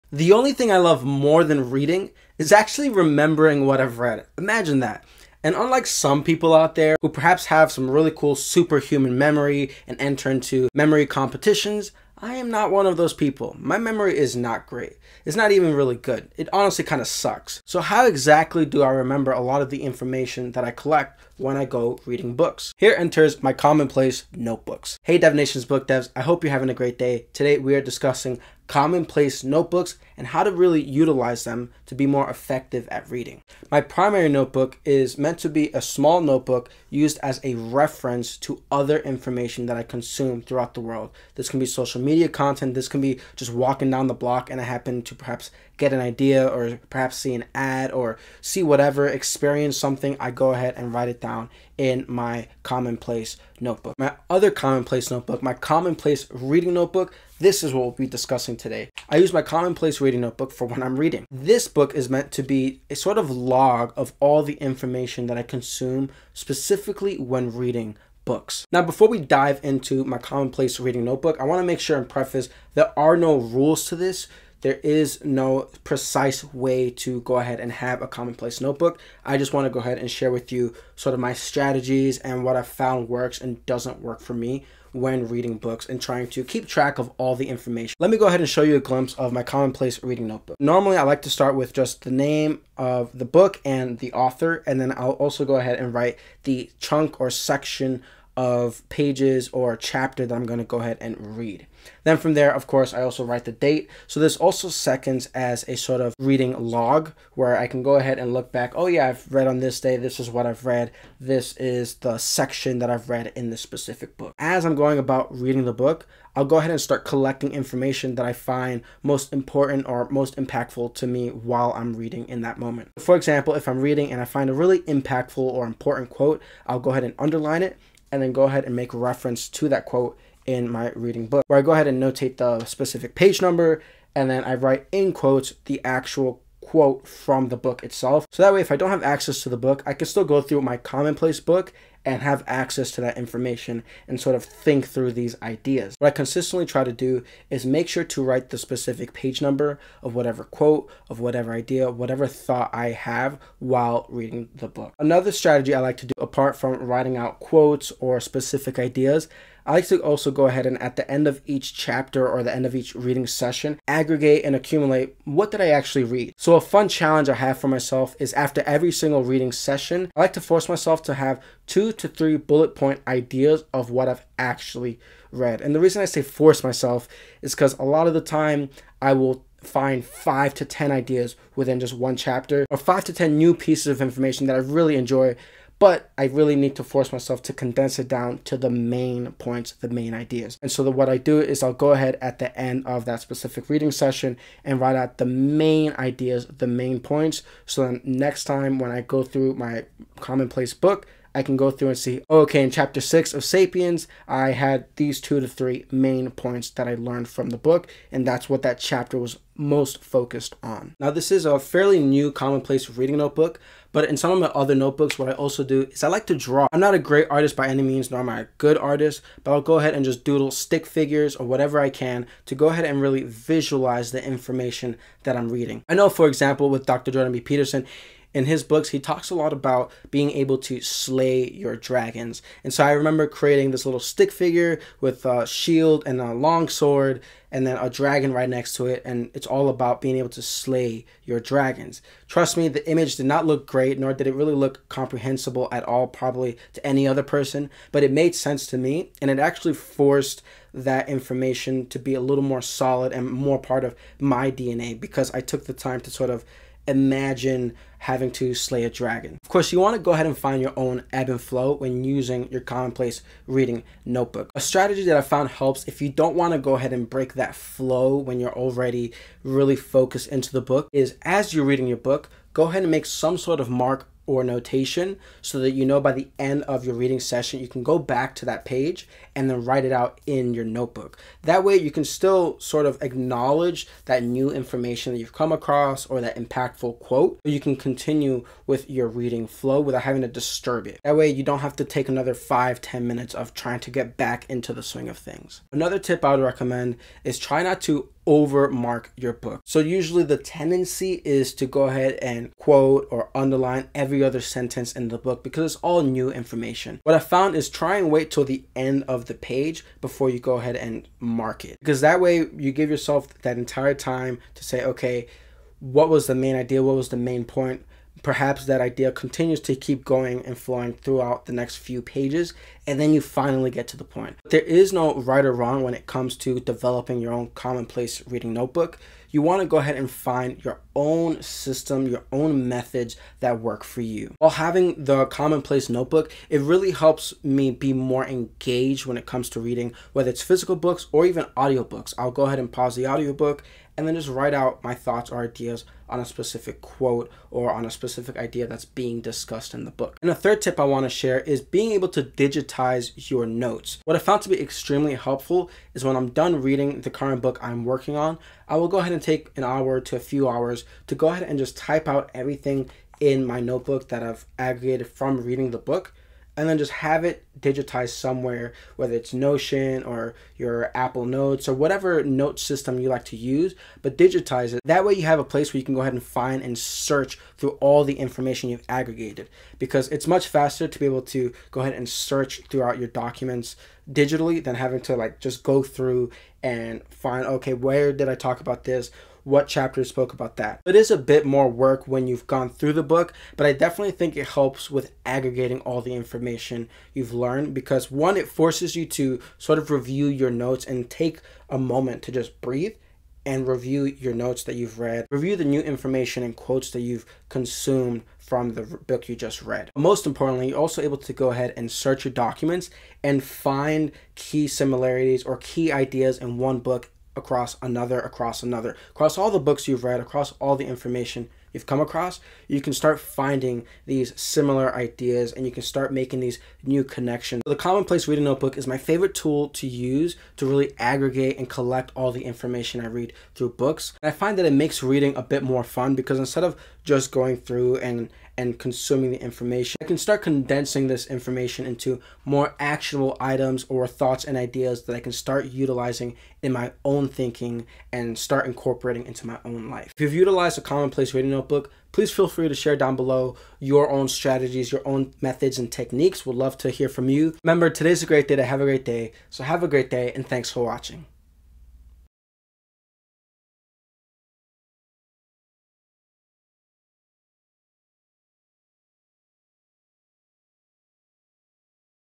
How I Use My Commonplace Book to Read More
Remembering what we read enhances the experience. Though while it sounds silly, remembering what we read is tougher than it sounds. If you love reading but struggle to remember what you've read, this video is for you! Discover how to effectively use a commonplace reading notebook to capture and retain valuable information from books. Learn practical strategies to enhance your reading experience and make it more engaging.

📚 Commonplace Notebooks: Learn the benefits of maintaining a commonplace reading notebook to log and reference important information from books.
✍️ Note-Taking Techniques: Discover effective methods for documenting quotes, ideas, and key points during your reading sessions.
🎨 Visual Aids: See how simple drawings can help solidify and recall important concepts.
💡 Personalization: Understand that there are no strict rules—find the system that works best for you.
💻 Digitization Tips: Get tips on digitizing your notes for easy search and cross-referencing across different books and topics.

Remember, today is a great day to have a great day! Happy reading!

Reach out! @Mind Devs /  @DevNations  /  @DevNations: BookDevs   /  @PerezTheDev

How I Use My Commonplace Book to Read Efficiently | Learn How to Read More Books